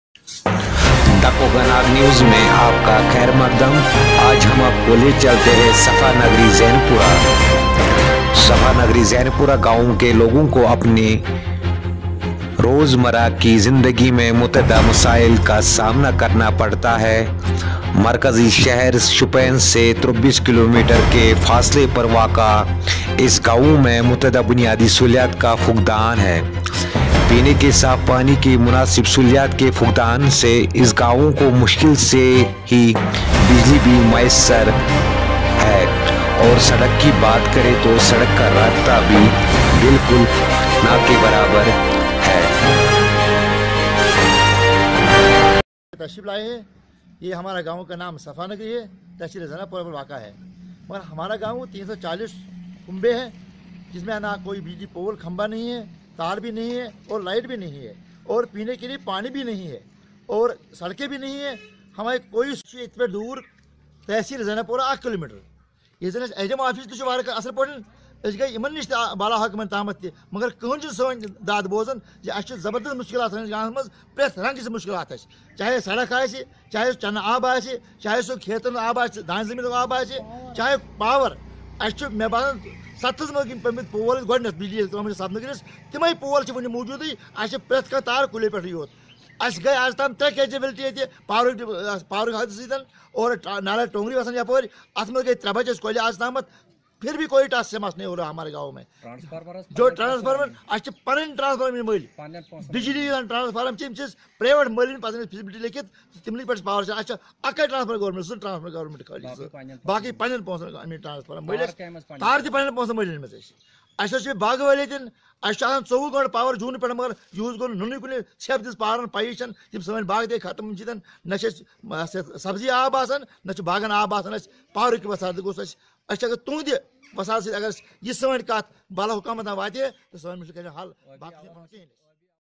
People in Safanagri Zainapora village face multiple problems in their day to day life.
People in Safanagri Zainapora village face multiple problems in their day to day life. Located at a distance of 23 kilometers from main town Shopian the village lacks multiple basic facilities. With a lack of proper drinking water facility, electricity is also hardly available to this village and road connectivity is bad.
report by umar yasin malik